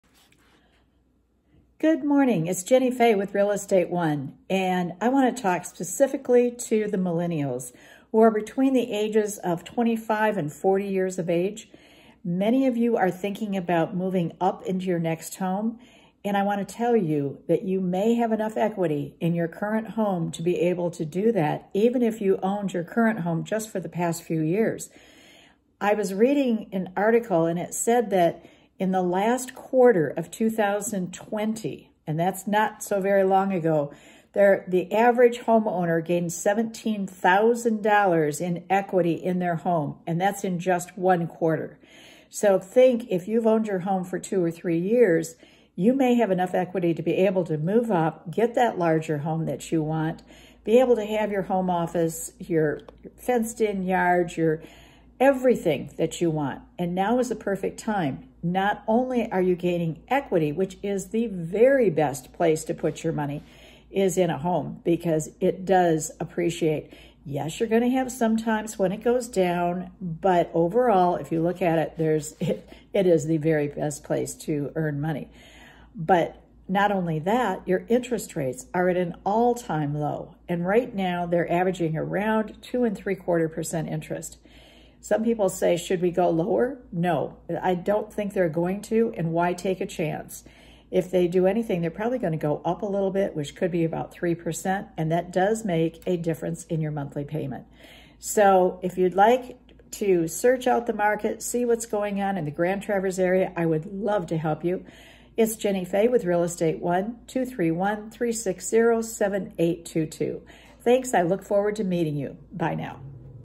Millennials, Is It Time to Buy a Bigger Home?
If you are a millennial homeowner between the ages of 25 to 40 that has been living in your current home for a while, do you realize you may have enough home equity to help you make a down payment on a bigger house? It's a great time to use that equity and get more house for your money while mortgage rates are low. I'm here to open that next door for you!